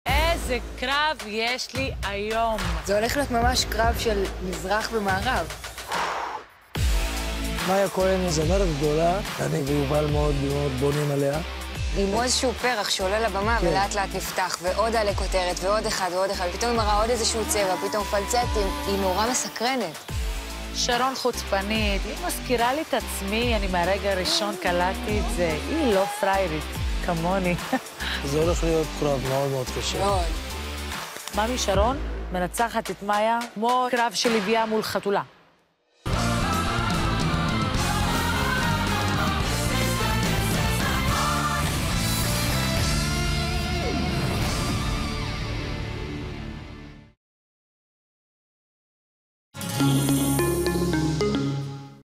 מי תנצח בקרב, מיה כהן או שרון דדוש?
קרב של מזרח מול מערב: מצד אחד מיה כהן מהנבחרת של שלומי ויובל, מצד שני שרון דדוש מהנבחרת של נסרין קדרי. מי תצליח להרשים את הקהל ותבטיח את מקומה גם בתוכנית הבאה?